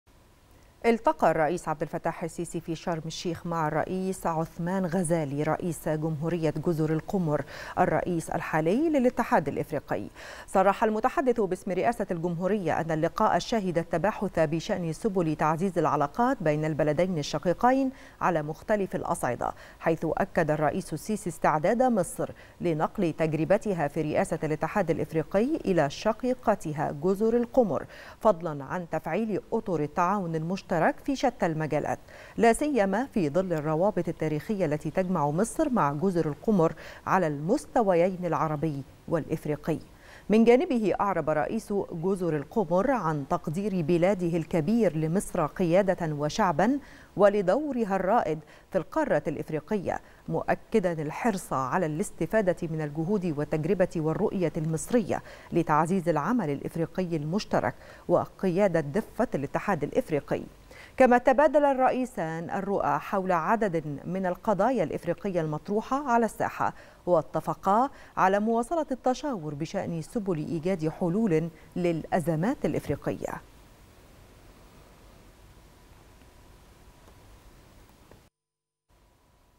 متحدث الرئاسة: الرئيس السيسي يلتقي بشرم الشيخ رئيس جمهورية جزر القمر الرئيس الحالي للاتحاد الأفريقي
نشرة الثالثة | متحدث الرئاسة: الرئيس السيسي يلتقي بشرم الشيخ رئيس جمهورية جزر القمر الرئيس الحالي للاتحاد الأفريقي عثمان غزالي